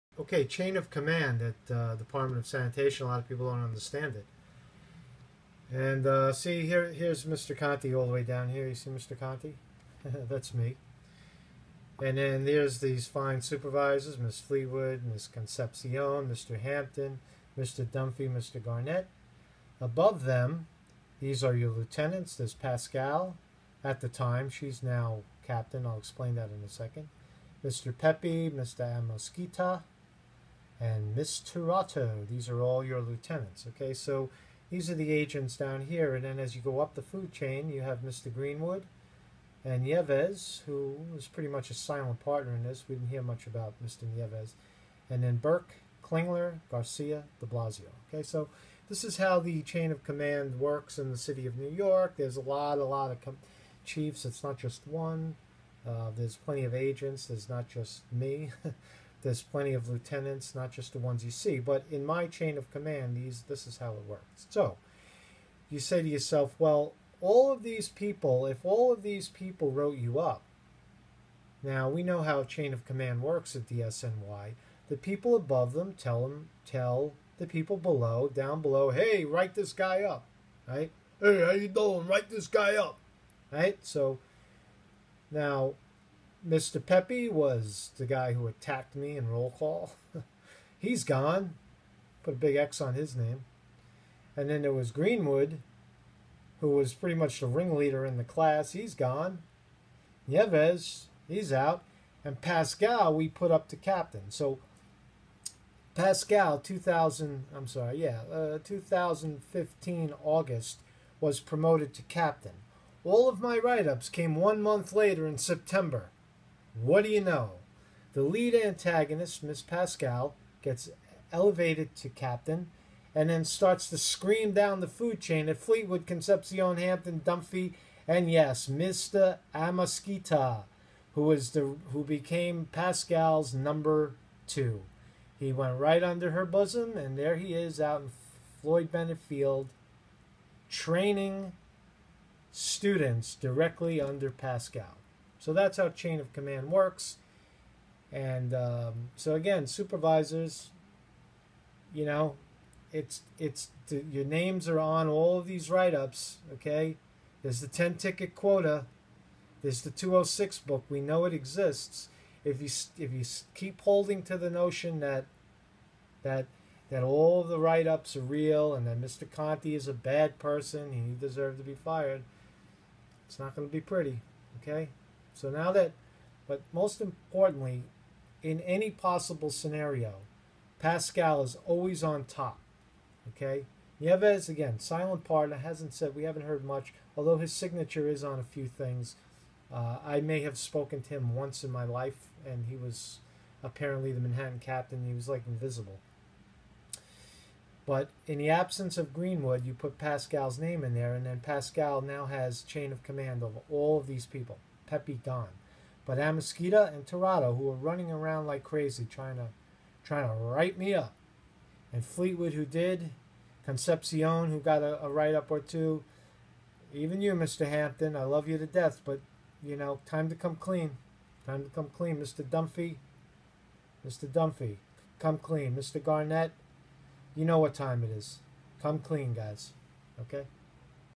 ConteGate: Naming Names Part TWO - Chain of Command DSNY Corruption
At DSNY, upper management makes ALL the decisions and drives them down the ranks. Orders flow downstream. Subordinate supervisors are NOT acting independently – but instead operate in a type of pyramid scheme. They do not make their own decisions. Through the use of force, intimidation, and threats, DSNY reduces their middle management to quasi-state actors who in turn use comparable force, intimidation, and threats on subordinate employees to achieve a goal. It’s a chain reaction. MAKE A CONTRIBUTION